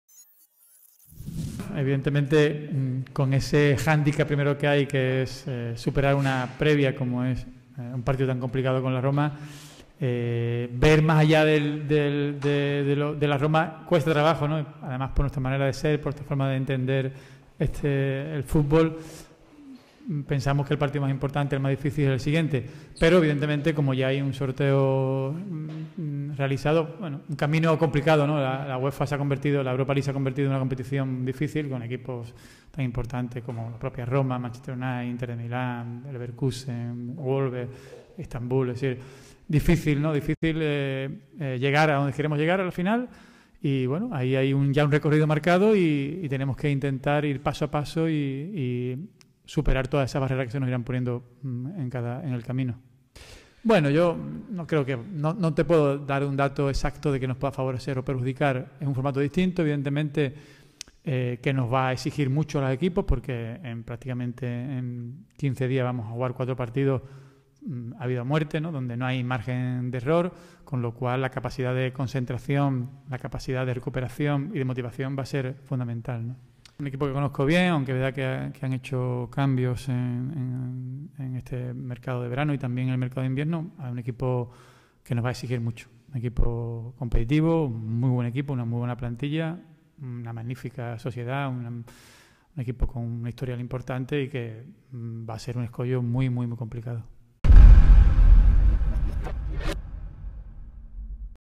Las reacciones de Monchi al sorteo de la UEFA Europa League
El director general deportivo analizó lo que le deparará al Sevilla FC la competición si supera a la AS Roma en octavos de final